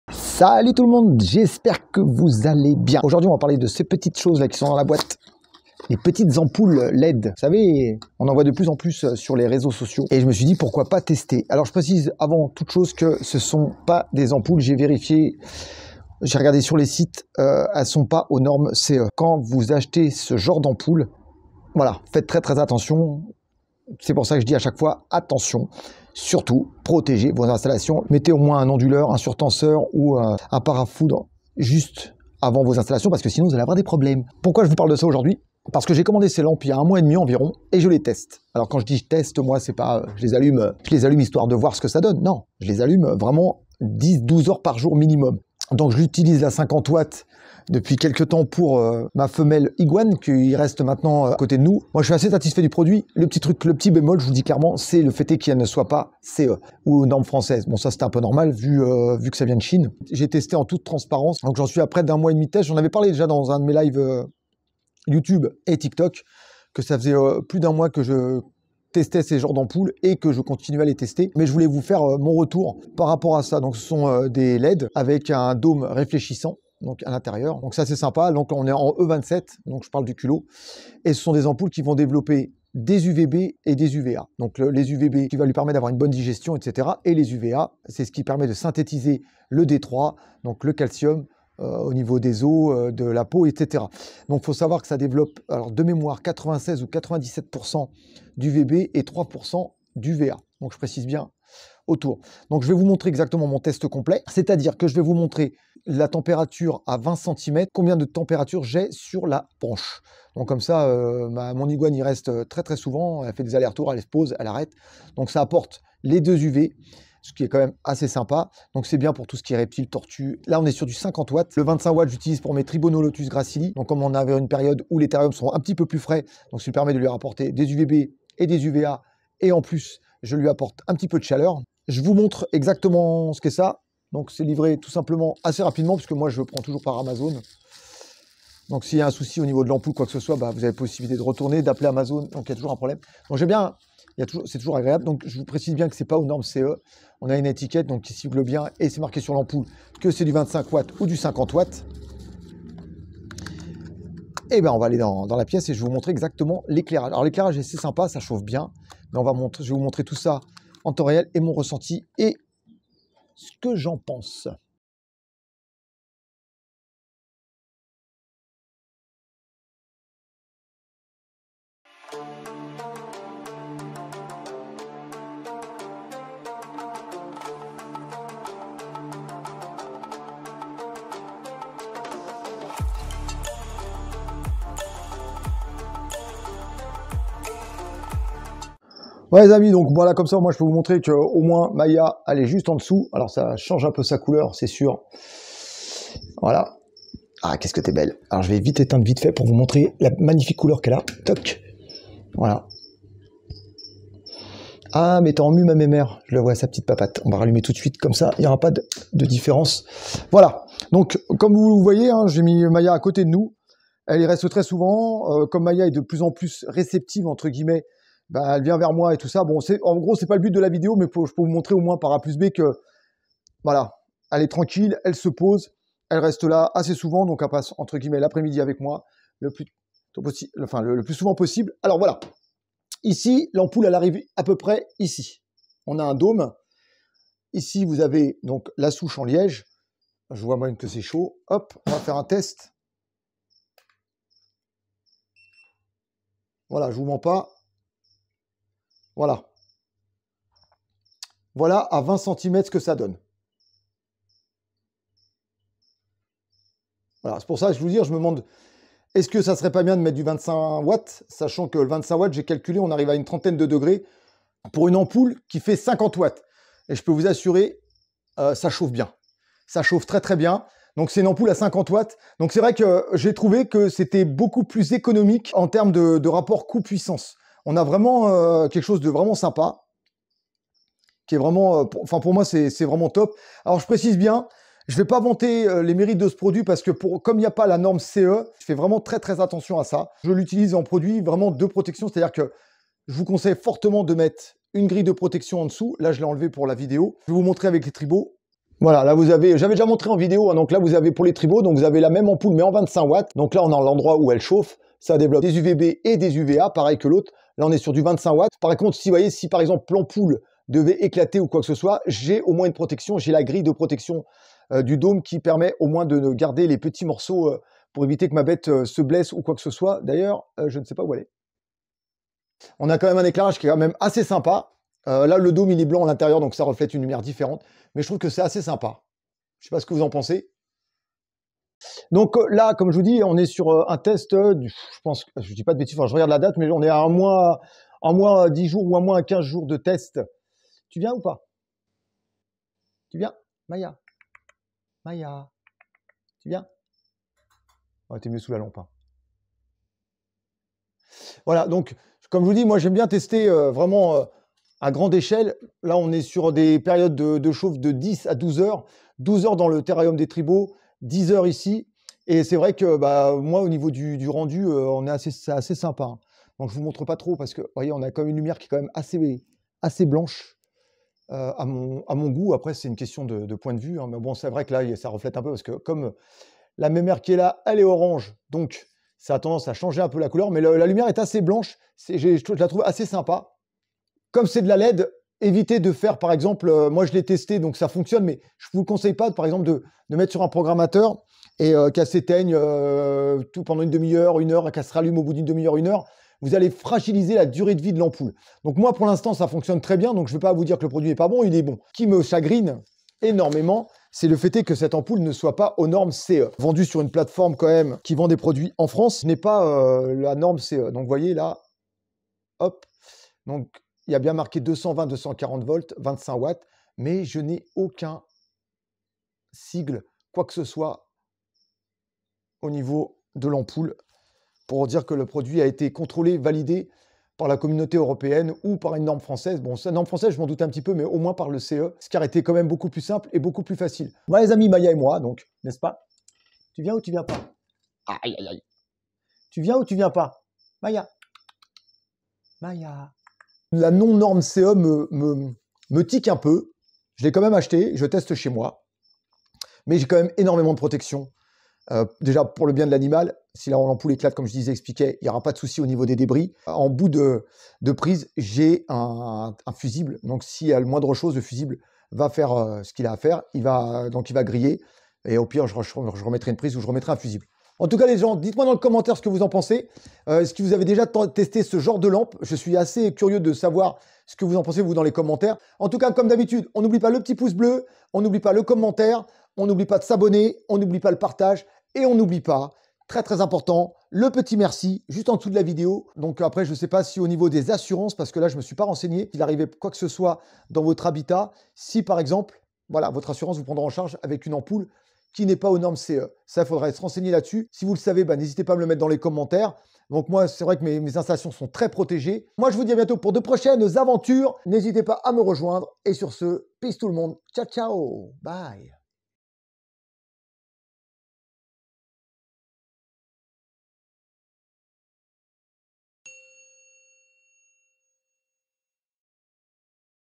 Mon avis sur les ampoules halogènes chauffantes UVB UVA d'Amazon
Dans cette vidéo je te donne mon avis sur les ampoules halogènes chauffantes UVB UVA. Attention aucunes normes légales.
Thermomètre sans contact laser
Multiprise parasurtenseur
Registre animaux non domestiques ▶️
T-shirt "j'peux pas j'ai terrario" ▶️